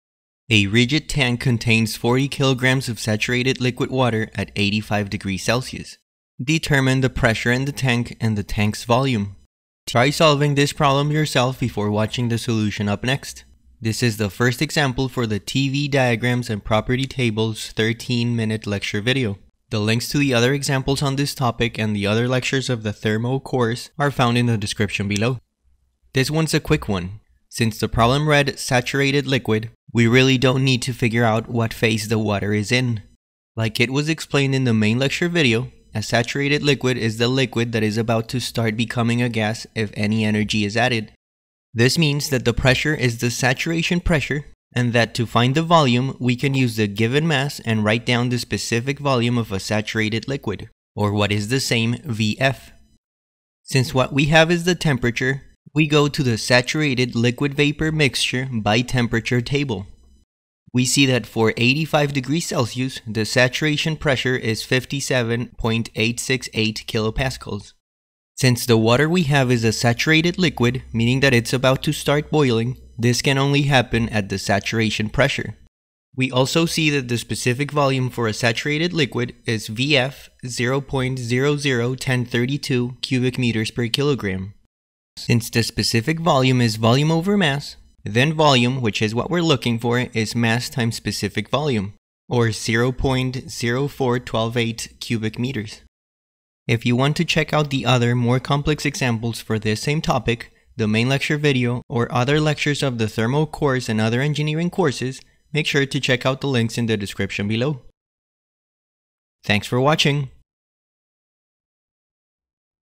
Thermodynamics Water TABLES: Saturated Liquid Example in 2 Minutes!
Saturated Water Vapor Mixture Table for Water  (no interpolation)

Other Thermodynamics Lectures:

28. Otto Cycle:
29. Standard Diesel Cycle:
30. Rankine Cycle: